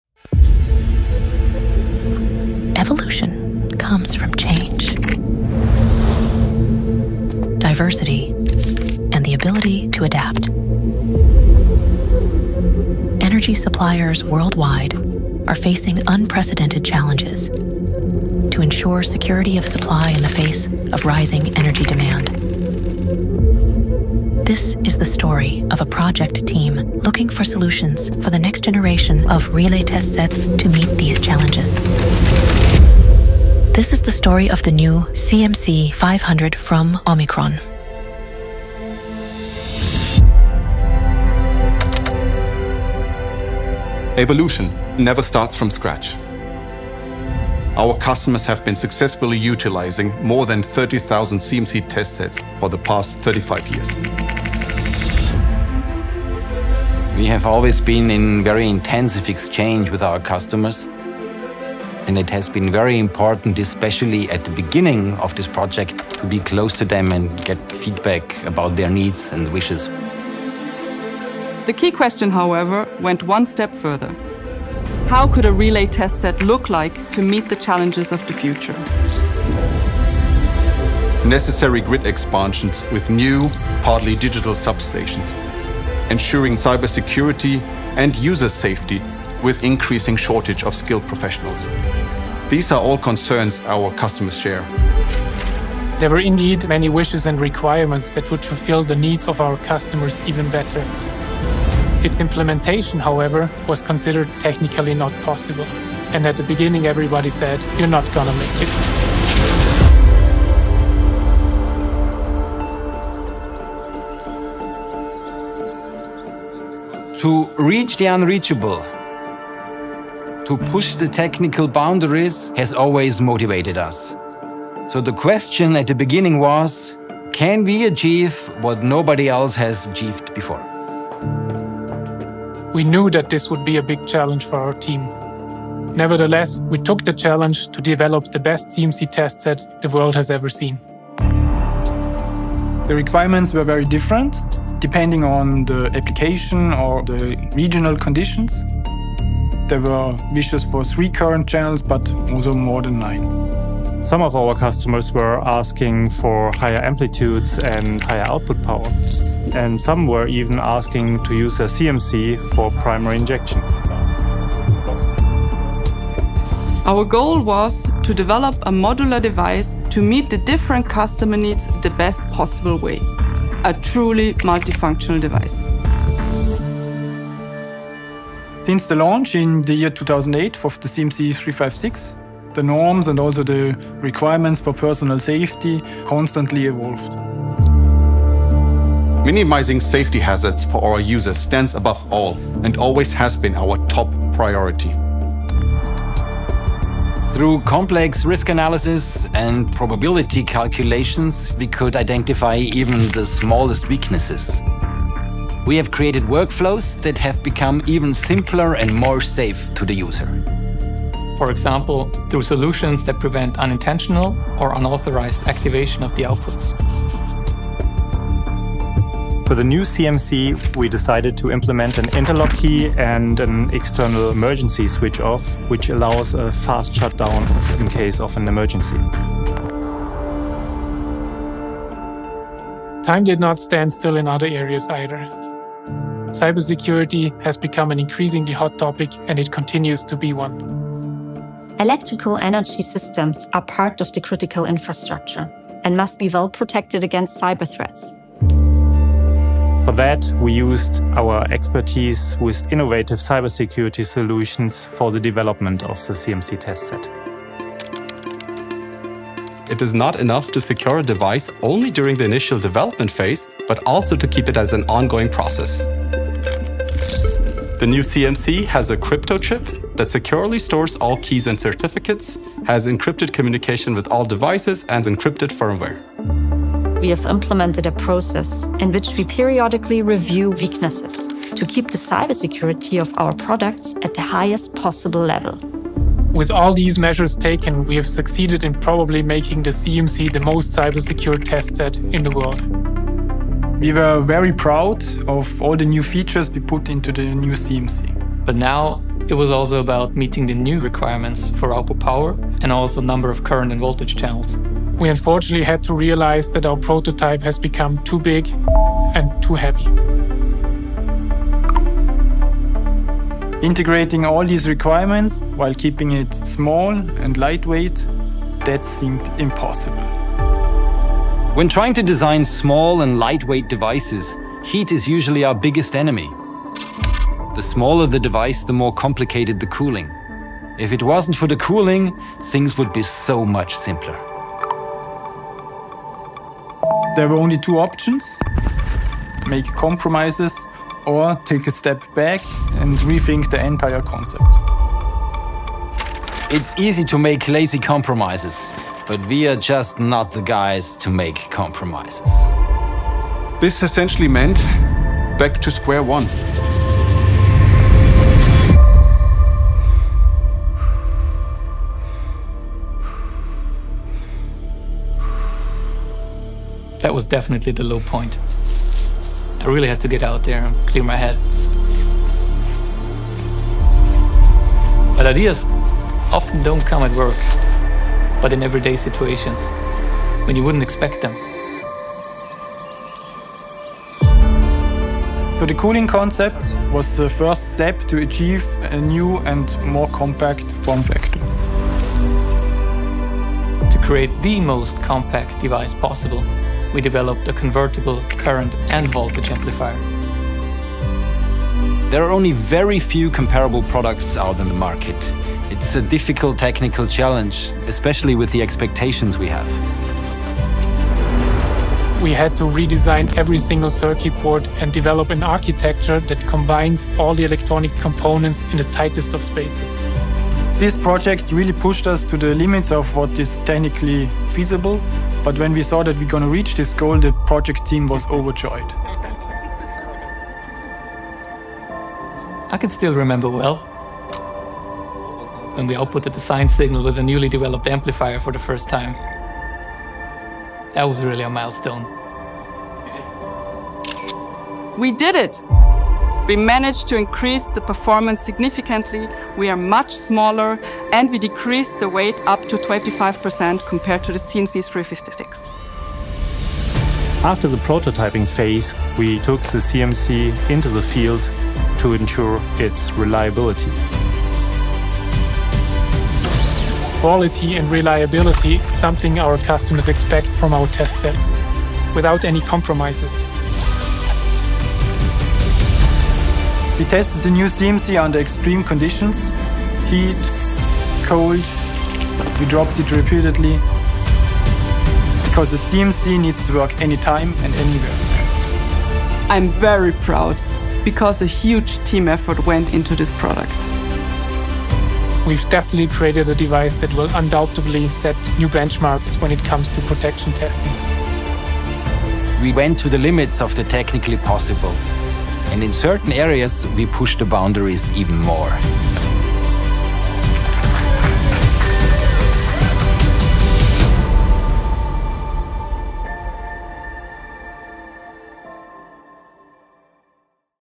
CMC 500 – The Story Behind the Innovation
The Story of the New CMC 500 - The New Benchmark in Protection Testing

👀 Here’s What You’ll See:
- The story behind our toughest challenges and how they shaped the CMC 500
- Real insights from our team on the hard work, innovation, and dedication behind the CMC 500
- Why the CMC 500 is built to meet your needs like never before

Ready to Experience CMC 500? 🎯
Be part of the journey and explore how CMC 500 can make a difference for you

Would you like to find out more about OMICRON and our products?